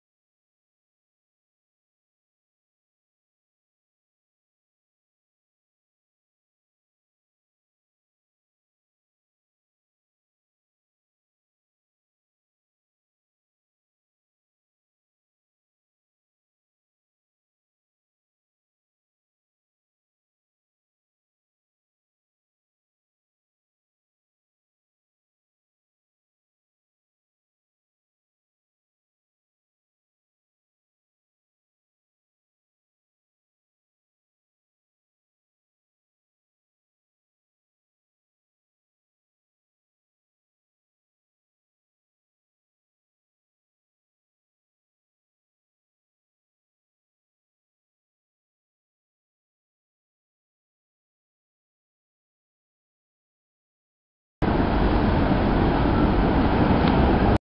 BNSF Manifest Train Passes Whitley Avenue
An Eastbound BNSF Manifest train passes Whitley Avenue in Corcoran on 4/2/2010